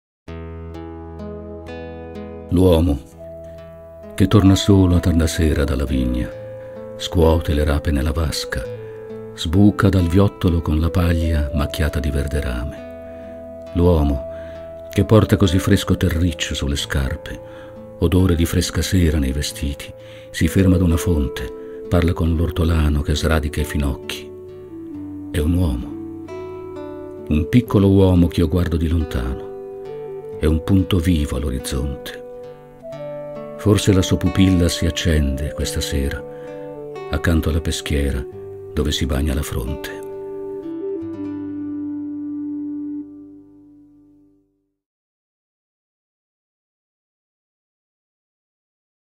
NANDO GAZZOLO – A MIO PADRE (di Leonardo Sinisgalli)
Poesia di Leonardo Sinisgalli – Lettore: Nando Gazzolo